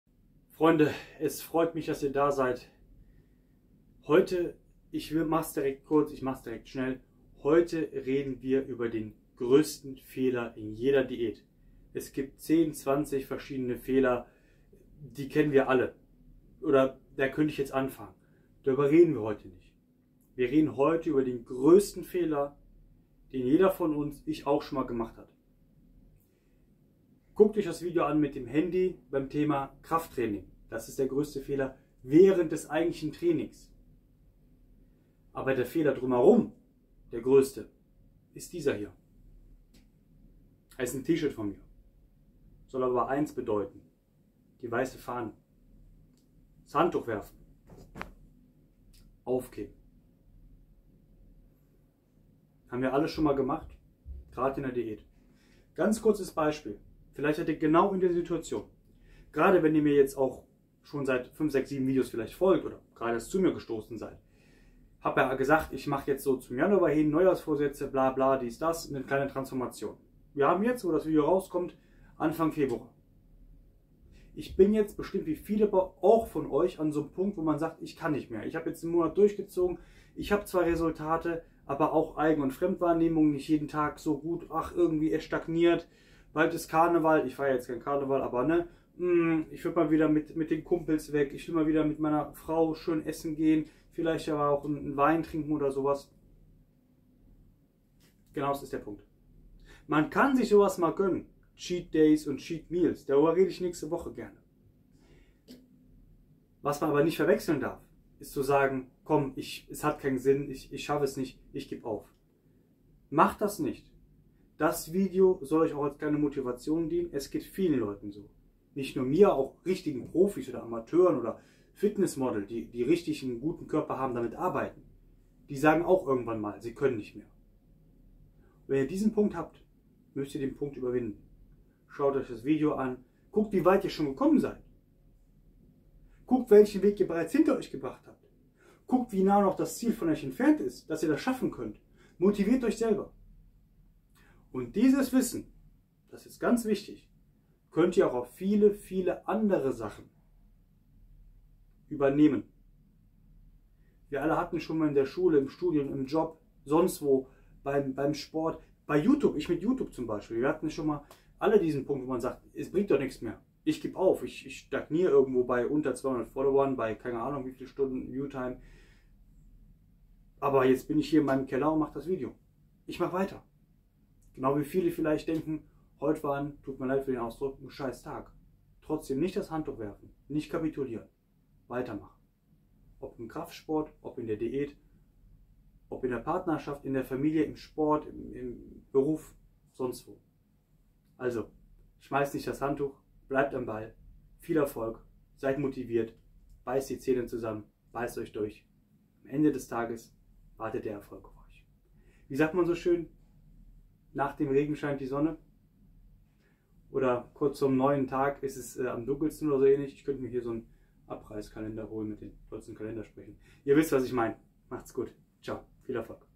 Der größte Fehler in der Diät!
00:00-00:30 Einleitung und Begrüßung
00:31-00:54 DER FEHLER
00:56-01:38 Beispiel
01:39-02:20 Bisschen Motivation
02:21-03:36 Wissen übertragen und Verabschiedung

Heute reden wir über den größten Fehler in er Diät: Das Aufgeben!